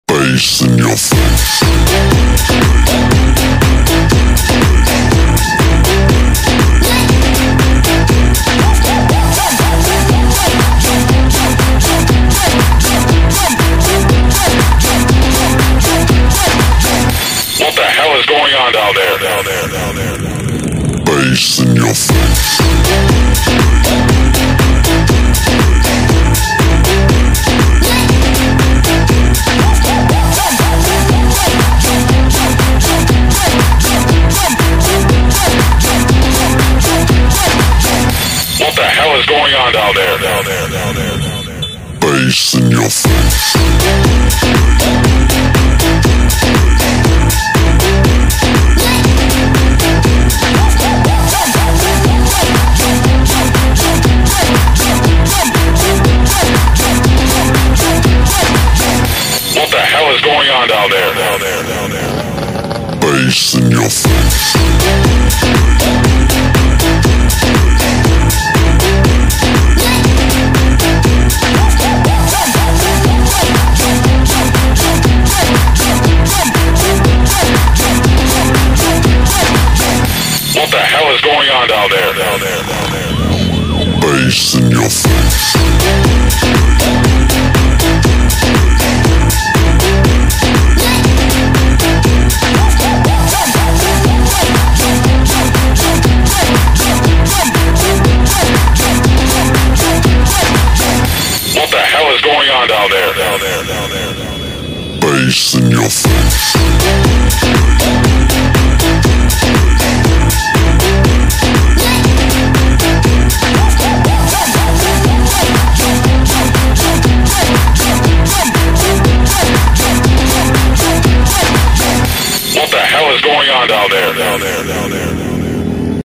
Konvoi Fans Belanda Di Kota Jayapura Papua Membuat Jalan Jadi Orange || World Cup 2022 Qatar
Konvoi Fans Belanda Di Kota Jayapura Papua, dalam rangka memeriahkan World Cup 2022 Qatar